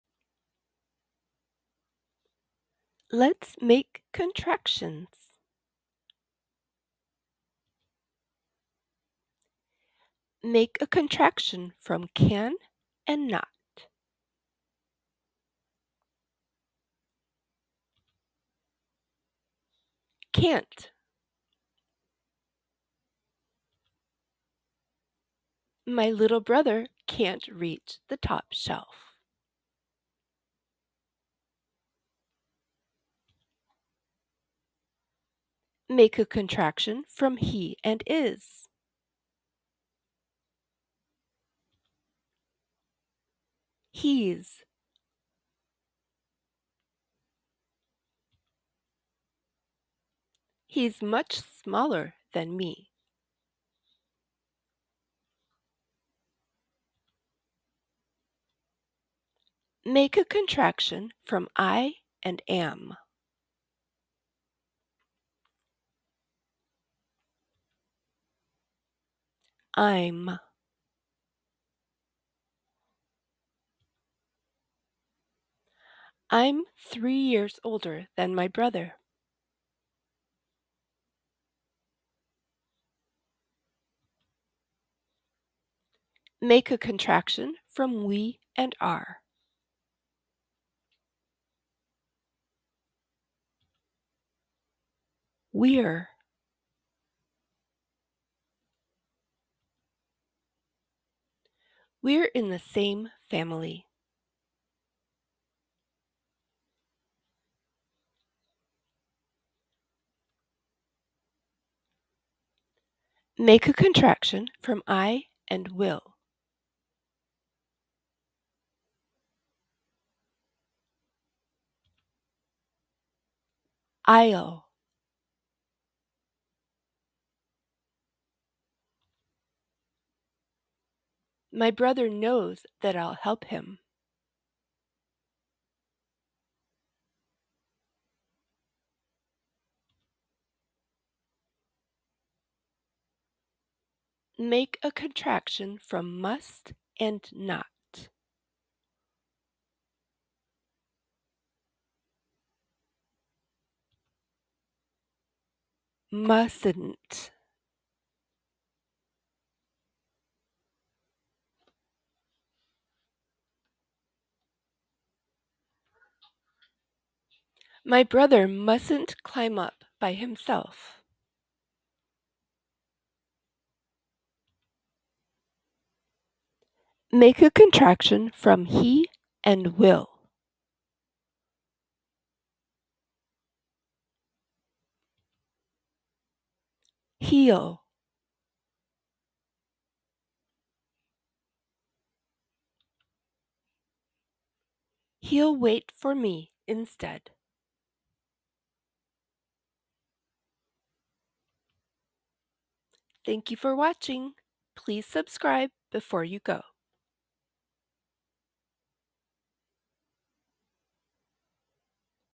Craft Contractions: Enhance Writing Skills | 3rd Grade Review
Practice making contractions.